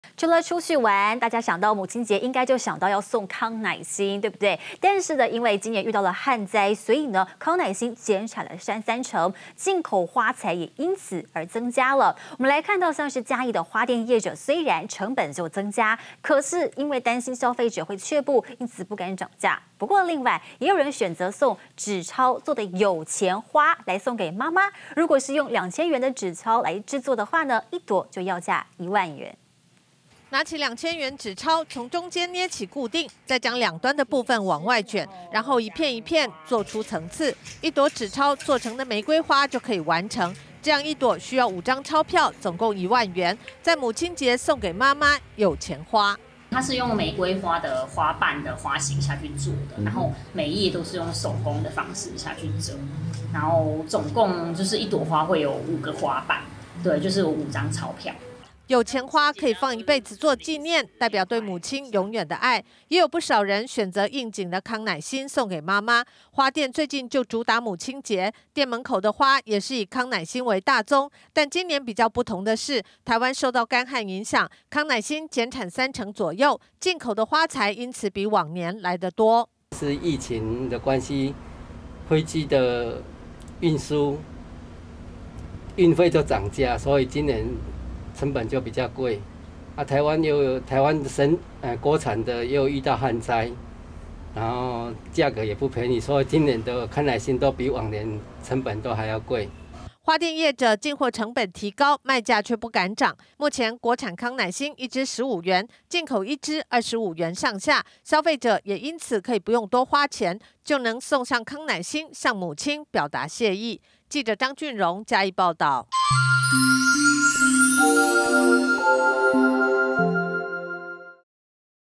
1朵1萬！ 紙鈔摺成玫瑰花束 母親節送媽媽「有錢花」
母親節不少人選擇送康乃馨向母親表達謝意，今年因為遇到旱災康乃馨減產三成，進口花材因此增加，嘉義的花店業者雖然成本增加，可是因為擔心消費者卻步，因此不敢漲價，另外也有人選擇送紙鈔做的「有錢花」送給母親，如果是用兩千元紙鈔製作，一朵就要一萬元，但價錢不是問題，因為當孩子的，就是要讓母親有錢花。